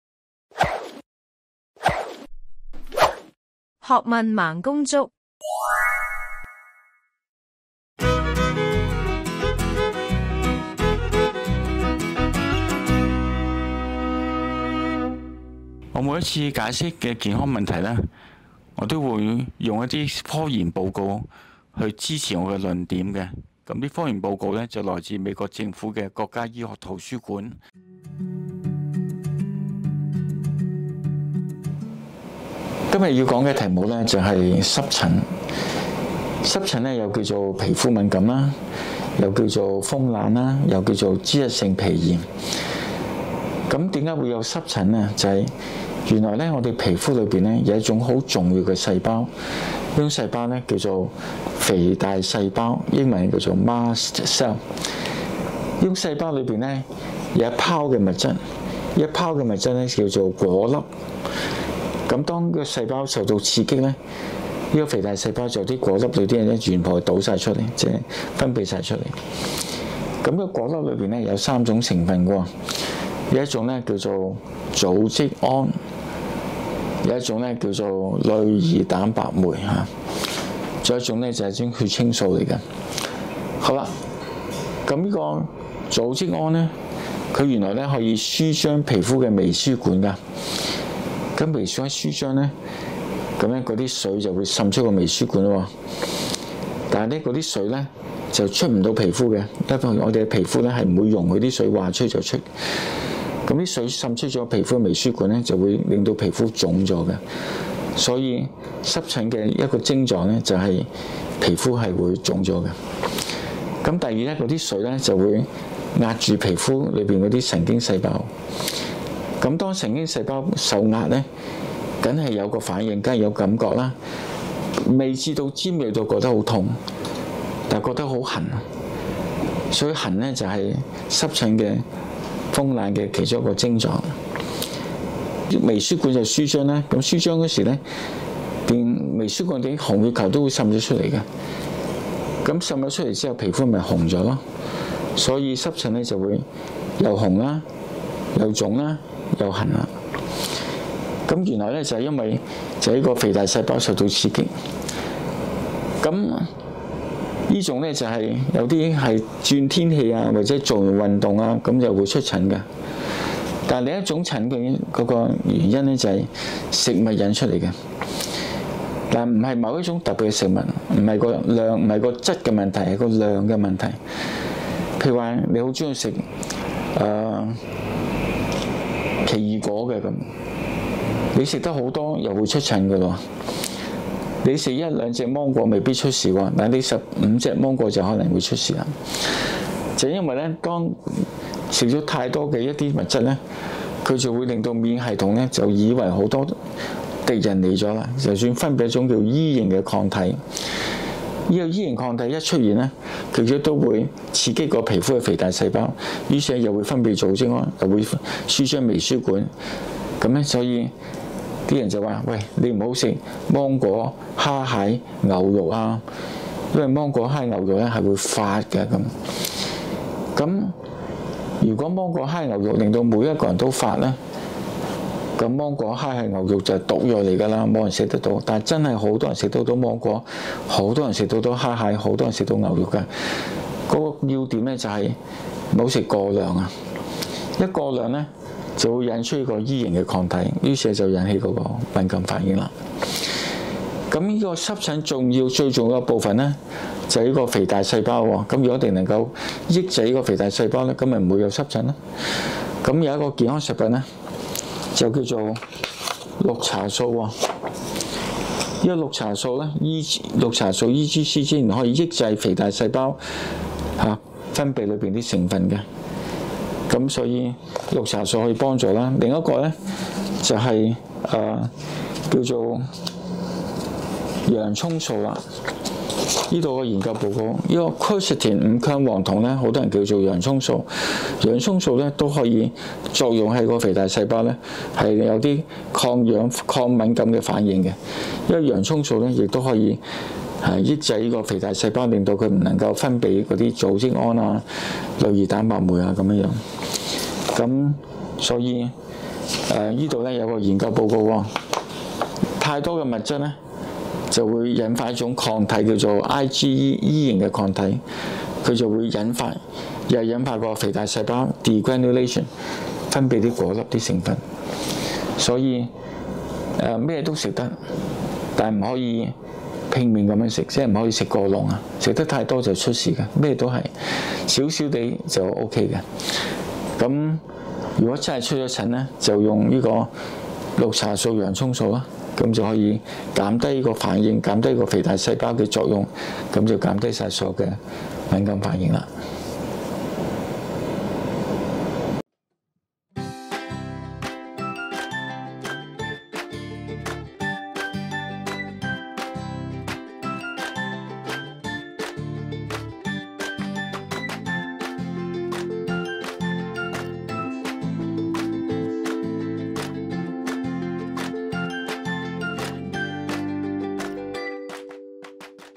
皮膚敏感 | 顧小培博士講解皮膚敏感成因及改善方法
顧小培博士地址及網址：
香港九龍尖沙咀東部麼地道62號 永安廣場UG24號舖 （地鐵尖東站P1出口）

營業時間: 11:30​ - 18:30​

*顧小培博士之店，只此一家，別無分店*